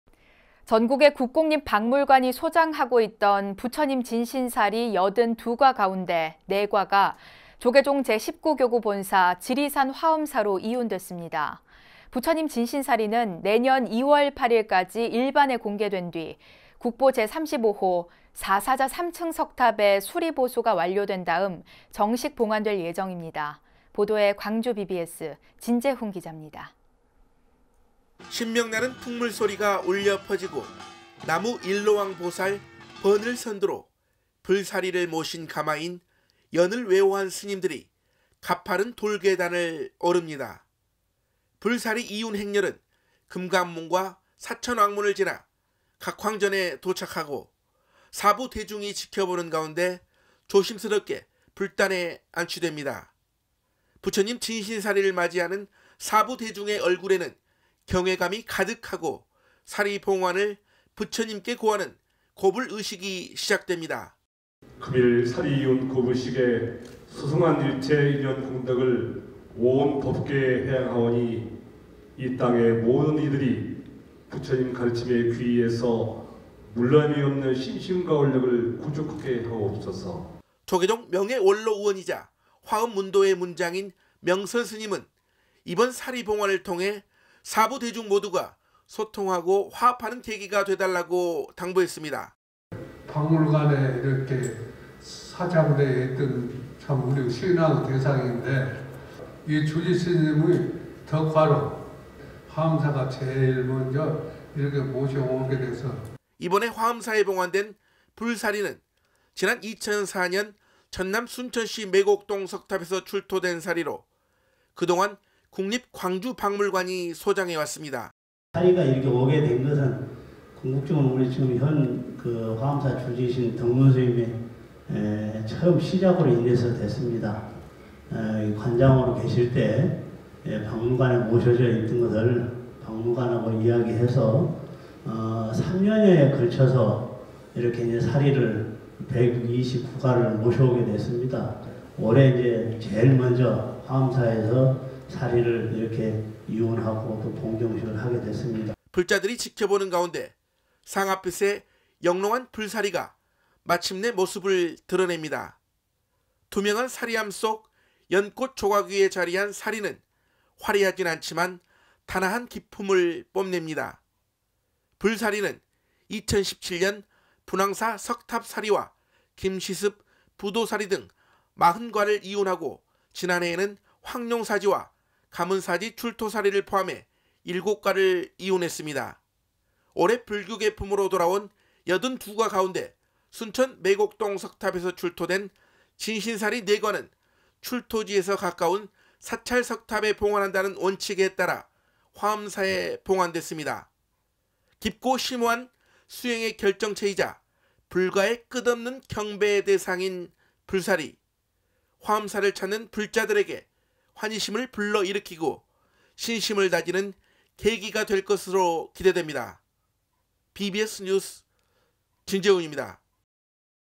[BBS뉴스] 수행의 결정체' 부처님 진신사리 화엄사 이운
전국의 국공립 박물관이 소장하고 있던 부처님 진신사리 82과 가운데 4과가 조계종 제19교구본사 지리산 화엄사로 이운됐습니다.

부처님 진신사리는 내년 2월 8일까지 친견 가능하며, 국보 제35호 사사자삼층석탑의 수리보수가 완료된 다음 정식 봉안될 예정입니다.

보도에 광주BBS 진재훈 기자입니다.